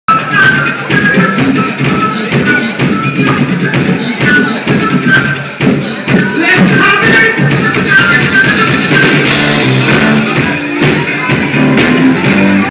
Traverse Flute
Traverse flute at the beach hotel in Byron Bay